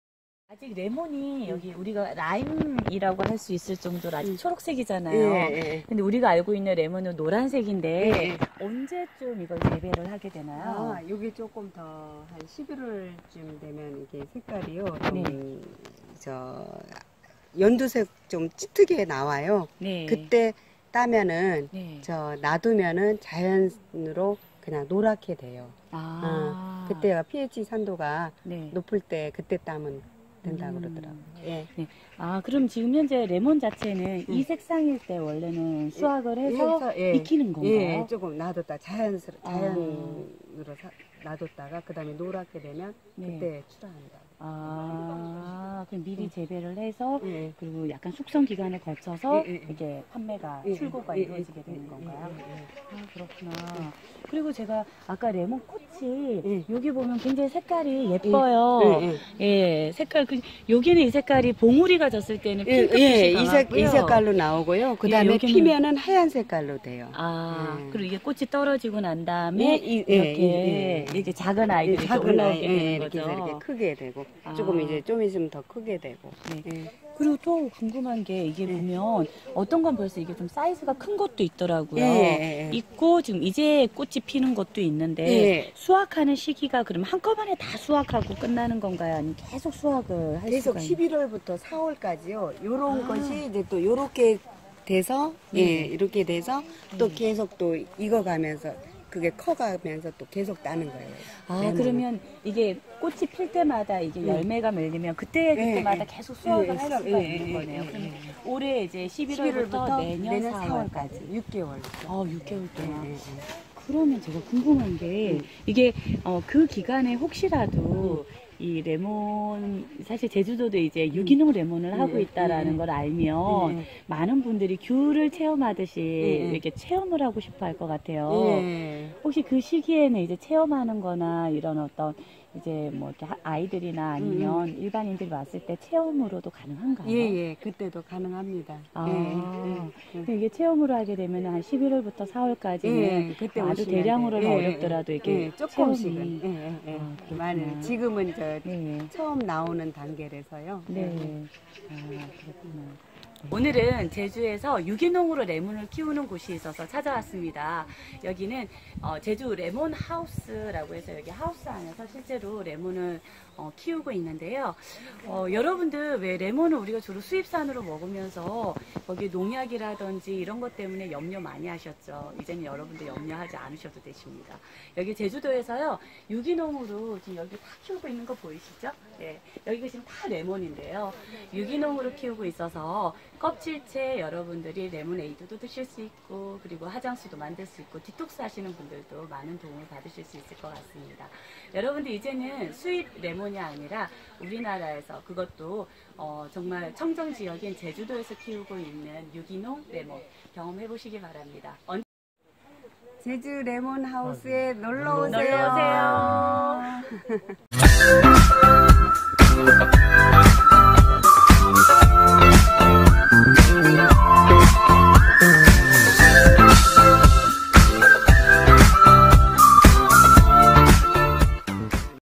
제주추천관광체험[Jeju Traveling] - 제주오가닉레몬 김복자의 레몬농사이야기2 by 농촌스토리텔링
제주여행하기 좋은 곳(Jeju Traveling)을 소개합니다.
제주쌈지농가에서 멋진 추억을 만드세요~

이야기가 있는 제주쌈지농가연합입니다~

농촌스토리텔링까페에 오셔서 확인하세요.